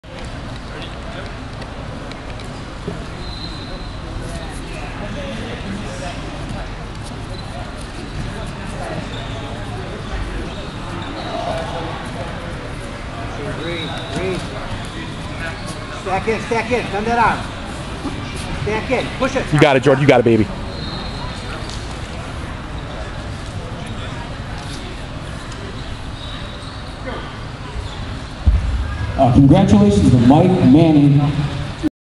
Prince Of Jiu-Jitsu Jordan Berry Hayastan Erie PA Flying armbar. Absolute division
20 second tapout of an unbeaten competitor in the absolute teen division. flying armbar;  semi final absolute